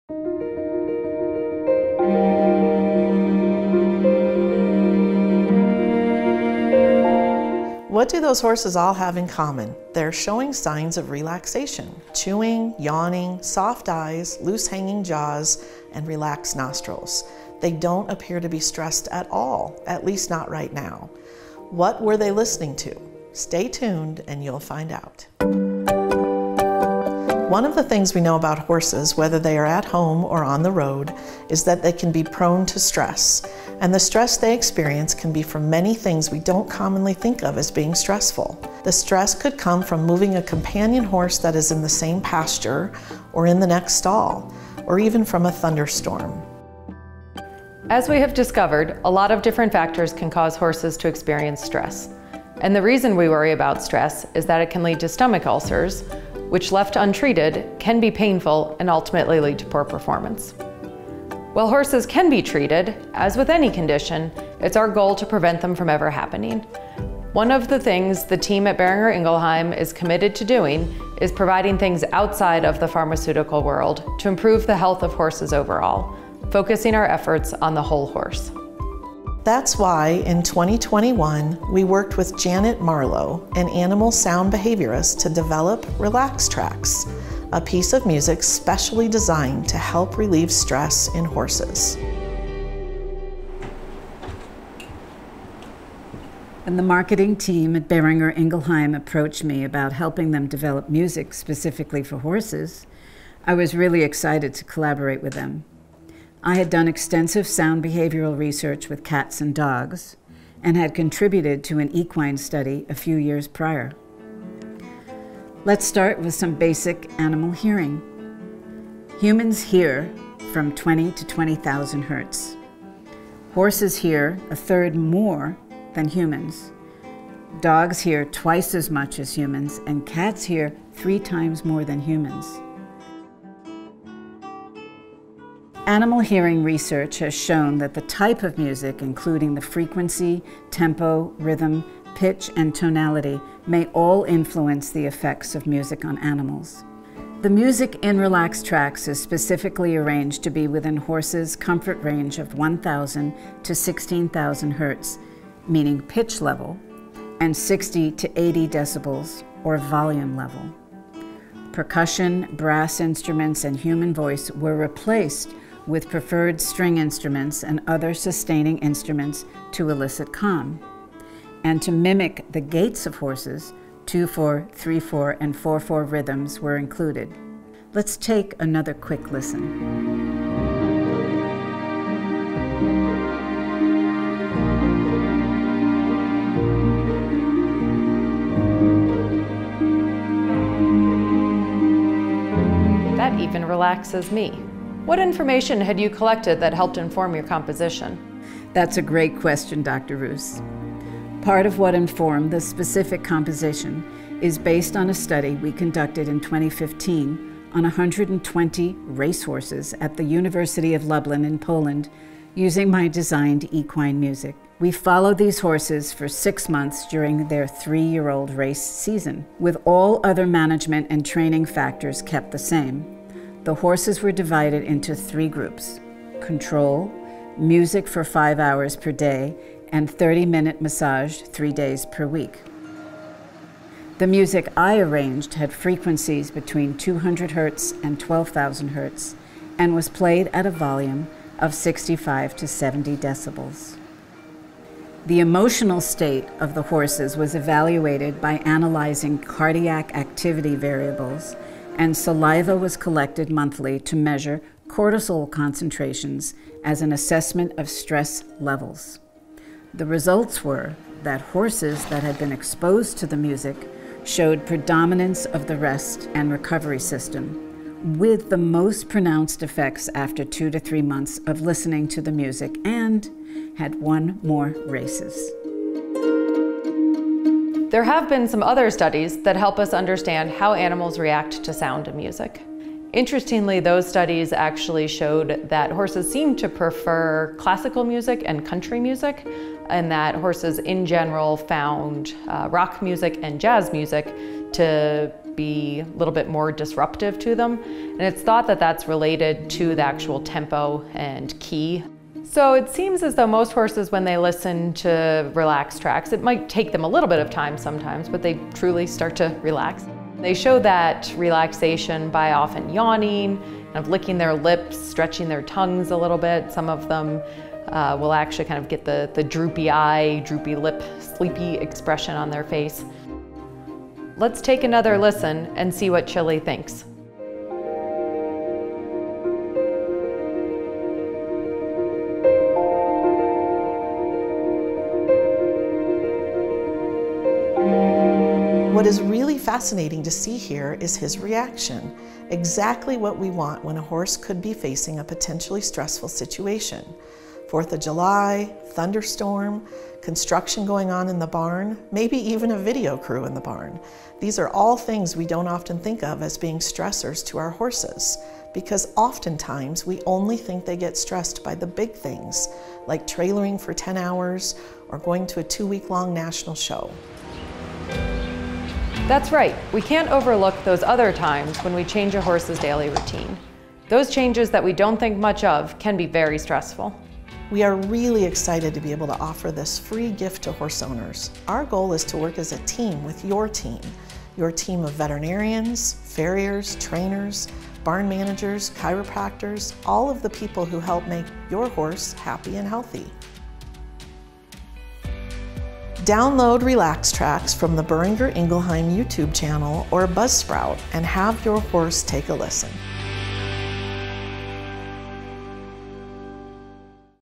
What's playing in the barn? A science-based approach to music​
Horses experience stressful events every day, both big and small. Prolonged periods of stress can lead to undesirable side effects such as equine gastric ulcer syndrome (EGUS).1 Relax Trax is specifically designed to relieve stress in your horse by mimicking the rhythm of a horse's gaits and using string instruments. Listen to animal sound behaviorist and creator of Relax Trax Janet Marlow talk about the science behind the music with Sarah Reuss, VMD, DACVIM, Equine Technical Manager at Boehringer Ingelheim and Kelly Graber, Associate Director Equine Marketing at Boehringer Ingelheim.

1McClure SR, Carithers DS, Gross SJ, Murray MJ. Gastric ulcer development in horses in a simulated show or training environment. J Am Vet Med Assoc 2005;227(5):775-777.